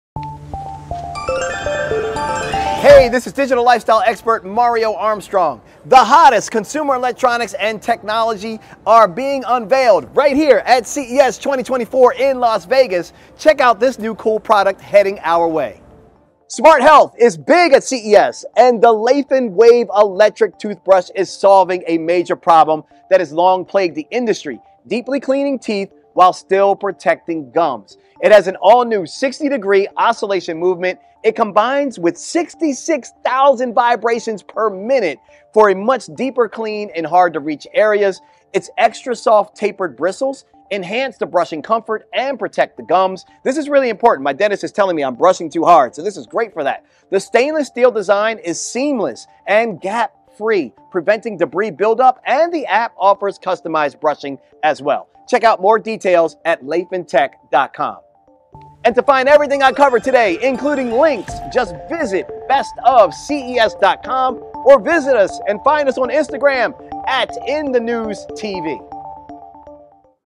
A new wave for everyone, meet Laifen Wave Electric Toothbrush
Hot debut at CES 2024, Laifen Wave became one of the trendy oral care techs and won acknowledgment from users, media, and professionals.
Delivers 60° oscillations & 66,000 vibrations/min, the Wave effectively solves the problem of cleaning teeth while protecting the gums and achieves a higher brushing efficiency compared to traditional electric toothbrushes. Futuristic, sleek, and ergonomic design for a more comfortable grip, providing an enjoyable brushing experience.

Repost from @inthenewstv